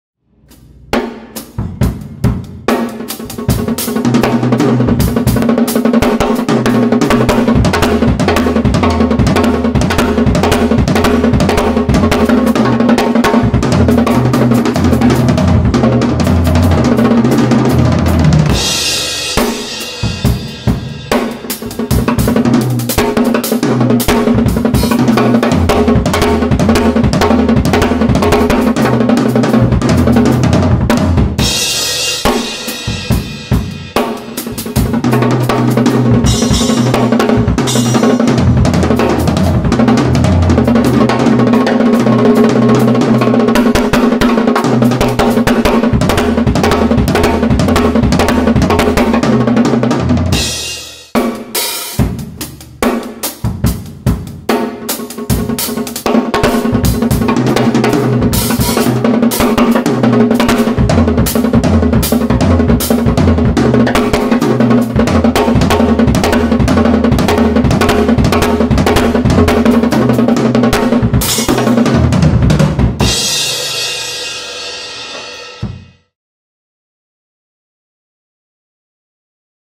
Linear drum fills - Looking for the killer sequence!
I discovered linear drumming quite late in my professional career, and although the licks are simple enough, it's not very easy to change your thinking and the sticking patterns you're used to. But I'm slowly getting the hang of it!